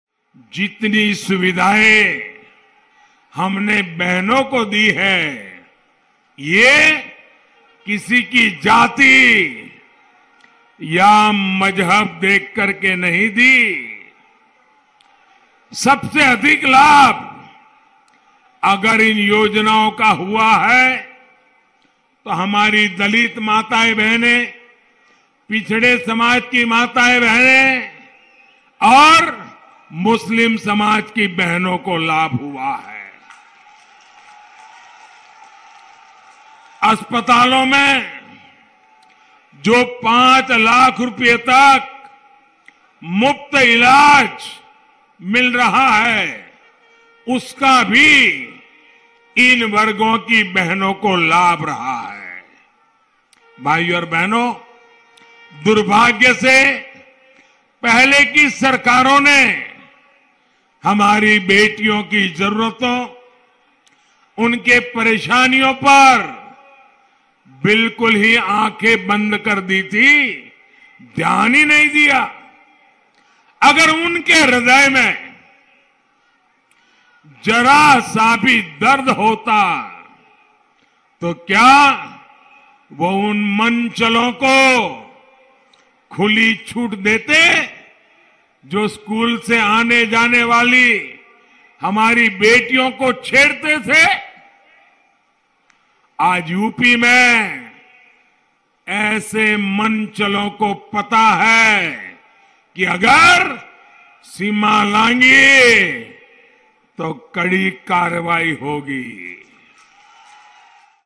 जितनी सुविधाएं हमने बहनों को दी हैं, ये किसी की जाति या मजहब देखकर नहीं दीं
सबसे अधिक लाभ अगर इन योजनाओं का हुआ है तो हमारी दलित, पिछड़े और मुस्लिम समाज की बहनों को हुआ है।

- पीएम श्री नरेन्द्र मोदी